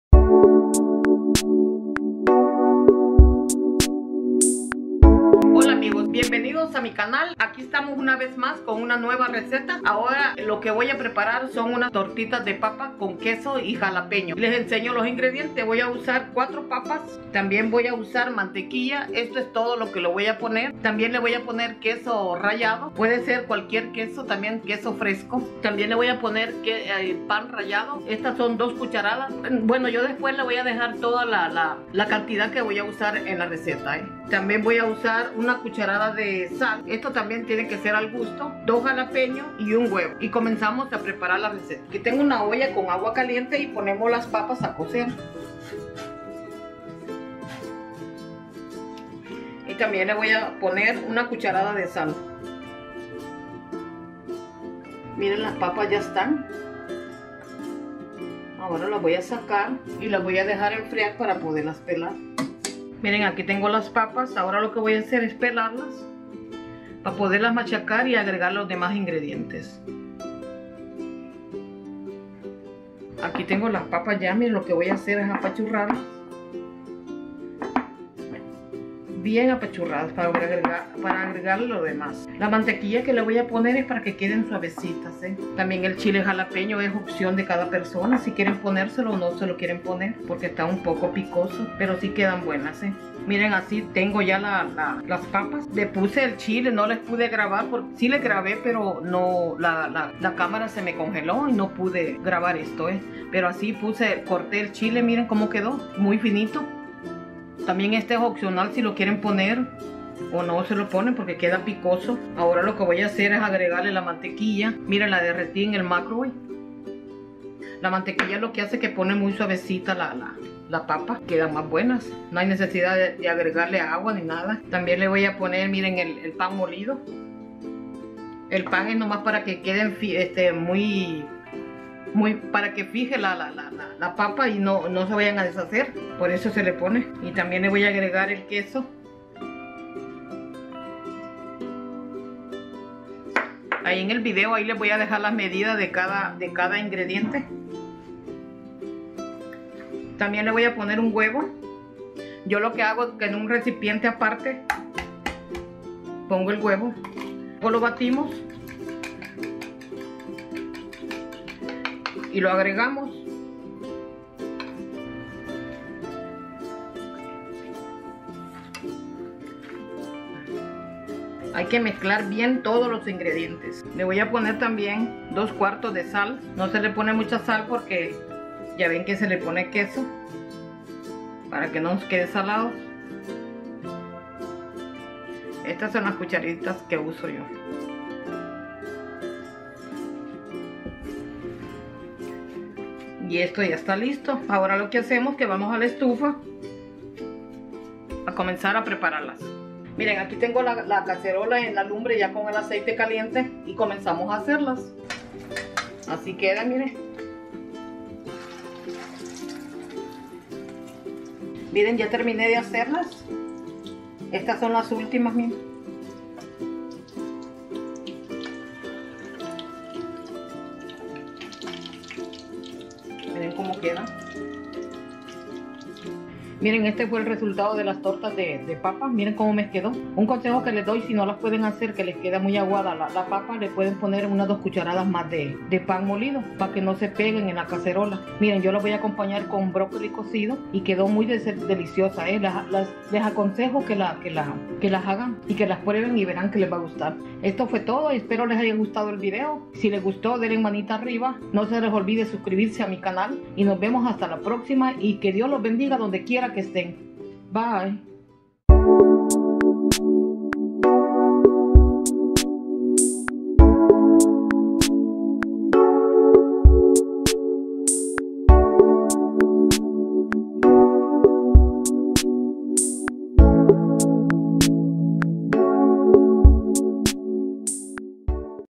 Tortitas De Papa Con Queso y Jalapeño/Potato Pancakes With Cheese And Jalapeno
ingrediente:
4 Papas
1/4 de Mantequilla
1/2 taza de Queso
2 cdas de Pan Molido
1 cda de Sal
2 Chiles Jalapeños
1 Huevo

ingredient:
4 Potatoes
1/4 of Butter
1/2 cup of Cheese
2 tbsp Ground Bread
1 tbsp of Salt
2 Jalapeno Peppers
1 Egg